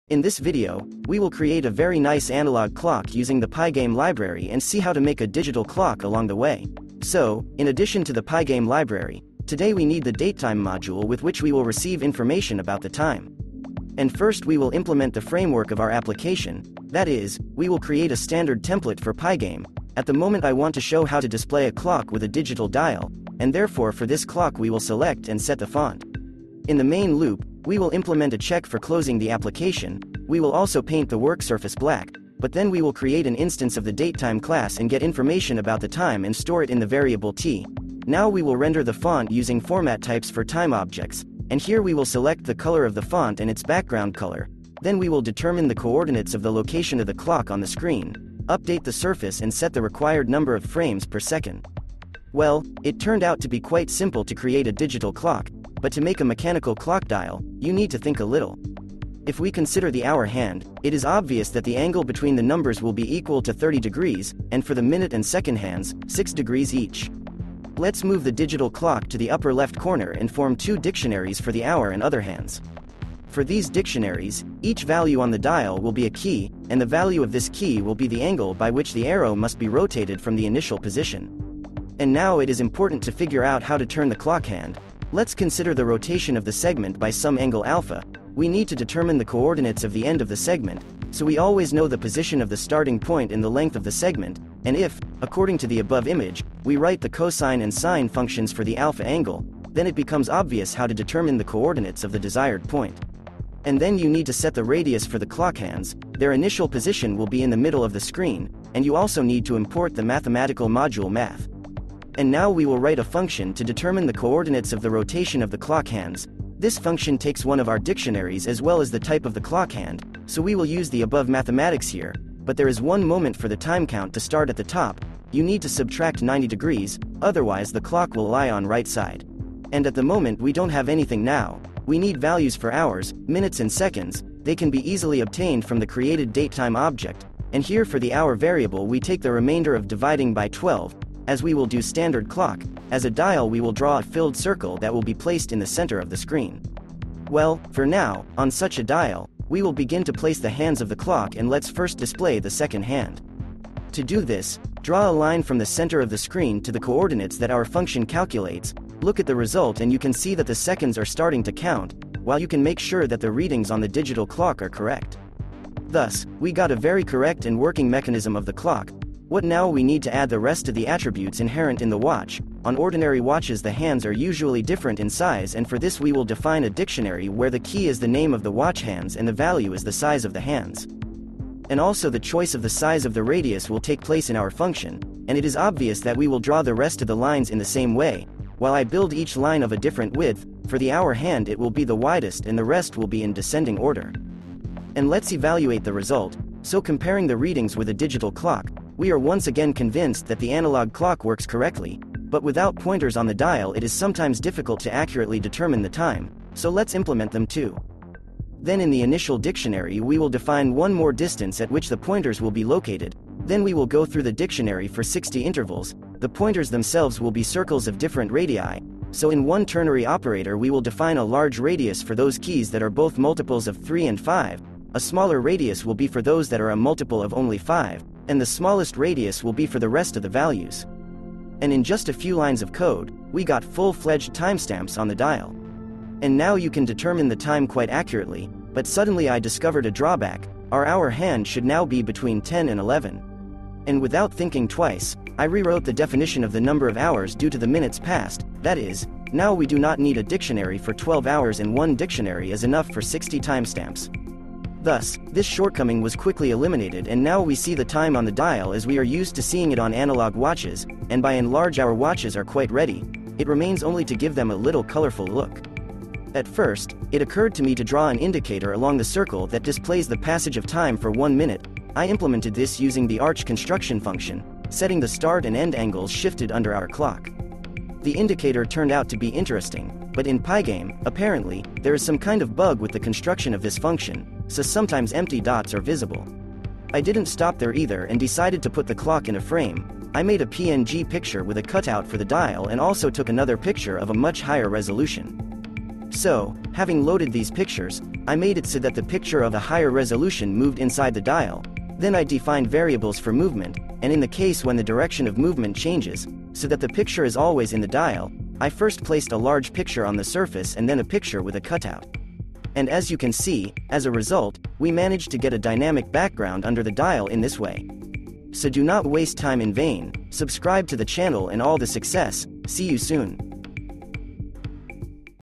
How to Make a Clock in Python
Pygame Tutorial on creating Analog and Digital clocks in Python.

BTC:
bc1qpmpk4swlwtj8kvwva5j7zlth6c33vzp8epx8w9

Any crypto on ETH or BNB:
0x855a59b4c926b96206e492774cd93d81287490ef

Any crypto on TON:
UQCnBXmaYswK9chnjw_KDPbf9uYGd6ZChexNc_iyLKllVKxd